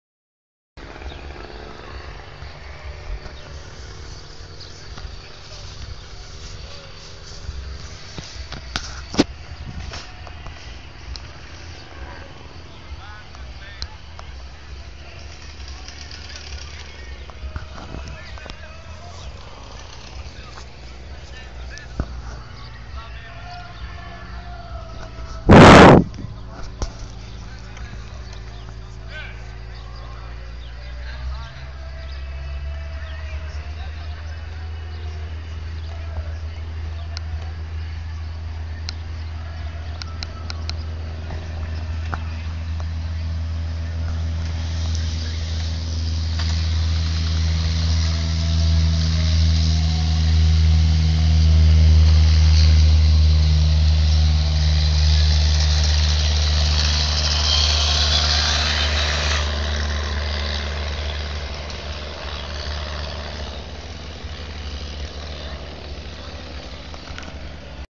Cessna 172 takeoff at LJCL (Levec)
Sorry for shaking my hand. I was just getting into position :D Enjoy !